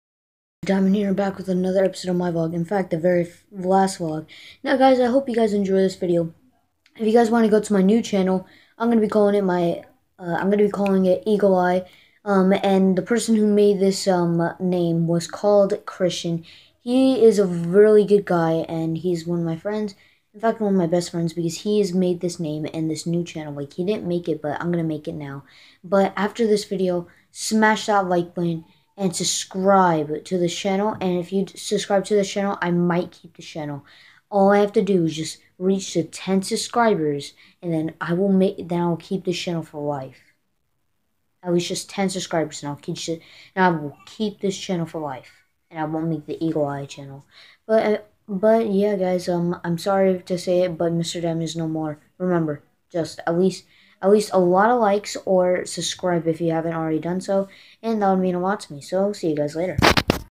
Mr_diamond might quit Youtube
Mr_diamond is going to not do youtube, but he will make a new channel called eagle eye